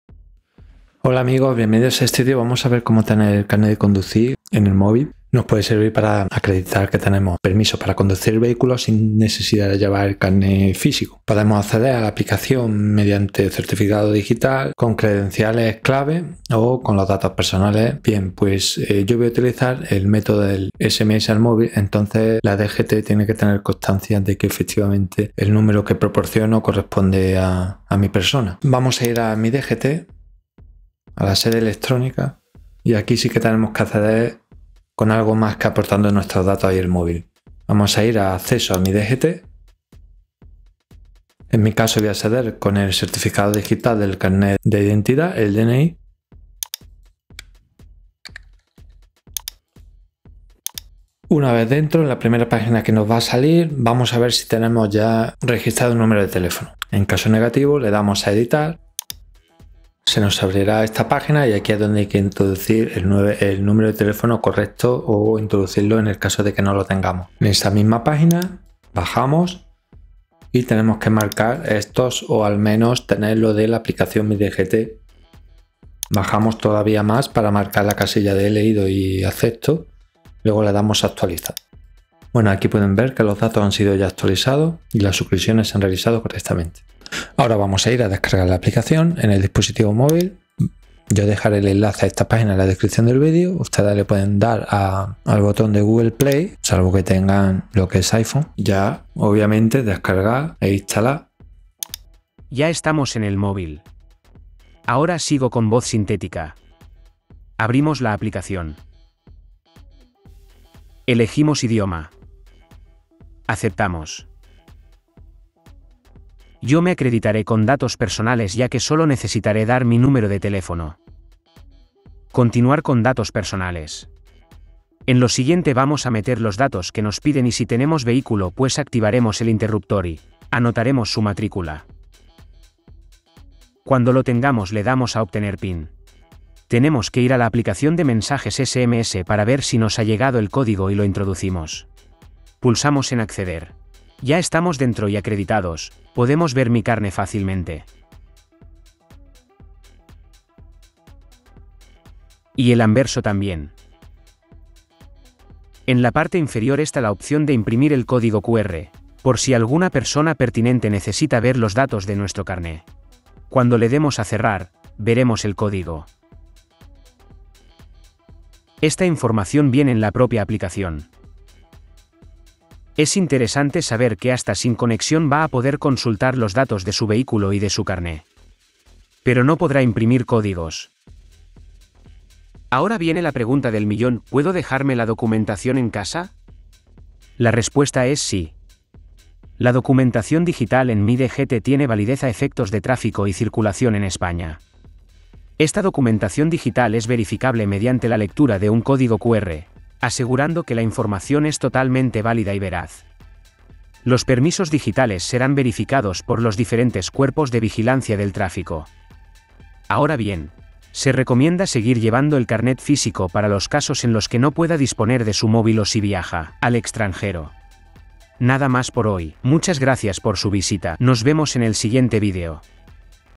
Cómo Tener El Permiso De Conducción en el MOVIL 📱
Recientemente la DGT (Dirección General De Tráfico) habilitó, lo que en resumen podría decirse, el llevar el carné de conducir en los smarphones. Aquí veremos cómo acceder a la aplicación miDGT para poder consultar, no solo los datos de nuestro carné, sino que, además, también los de nuestro vehículo. Debemos saber también que la aplicación nos vale para acreditar ante los funcionarios pertinentes tanto la documentación del vehículo (Permiso de circulación y datos técnicos) cómo la de nuestro carnet de conducir.

En este tutorial se explica cómo usar una aplicación en el móvil para tener el carné de conducir electrónico. Se puede acceder a la aplicación con un certificado digital, credenciales clave o datos personales, pero en este caso se utiliza el método de SMS. Primero, es necesario registrar un número de teléfono en la sede electrónica de la DGT con un certificado digital o el DNI. Luego, se descarga la aplicación en el móvil y se accede a ella con los datos personales y el número de teléfono. Una vez dentro, se pueden ver los detalles del carné y su información es verificable mediante un código QR. Aunque la documentación digital es válida a efectos de tráfico y circulación en España, se recomienda seguir llevando el carné físico en caso de no tener acceso al móvil o si se viaja al extranjero.